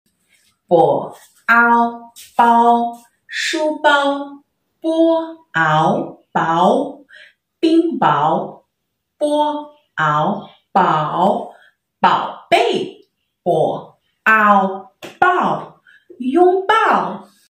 Chinese word conjugation using letter b+ a --- o (bao)
In this class you will learn easy Chinese from level zero, syllable by syllable
In the following link, to clip or copy and paste in your browser, and it will take you to my mandarin classes. Contains a Discount.